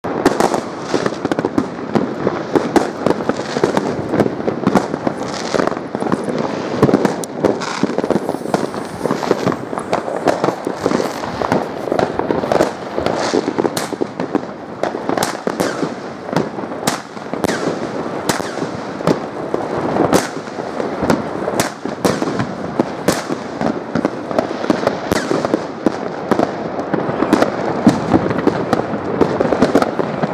Snake - Pyrostar
Fantastische mooie 7 shots cake van Pyrostar.
Effect: Silver spinner to multicolor blink.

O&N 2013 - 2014